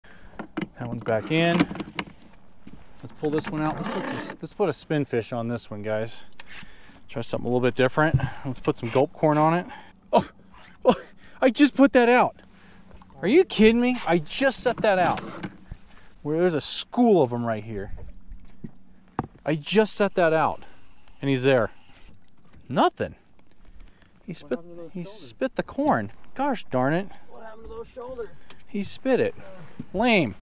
The Fish SPIT IT!!!
Just when you thought the kokanee had your bait really good...